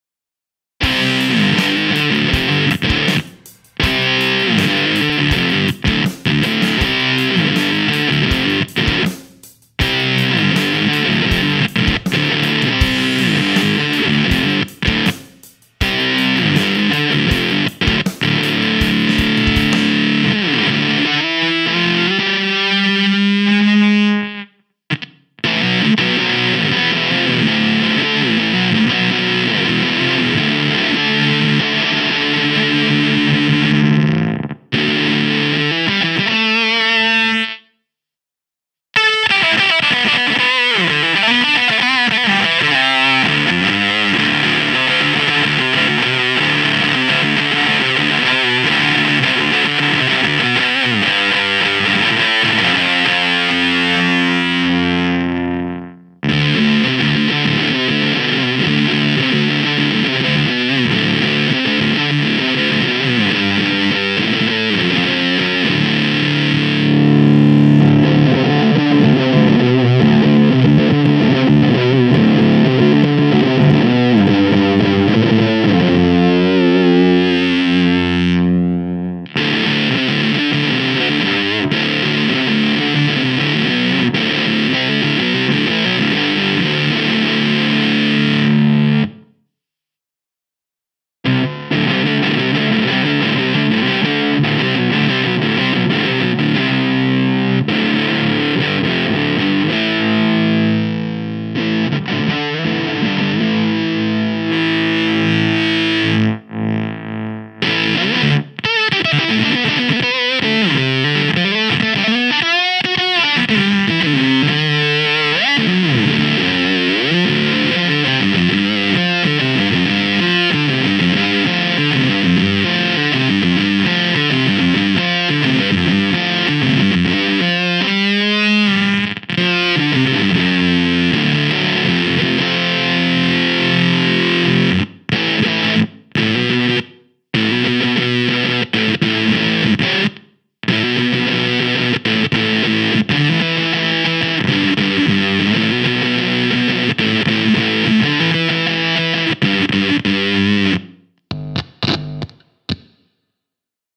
Way Huge Conquistador Fuzzstortion Demo
Though it has less 'wow' factor than some other fuzzes, the gated conquistador fuzz records really nicely, and when set right allows you to get some thick as fuck fuzz tones.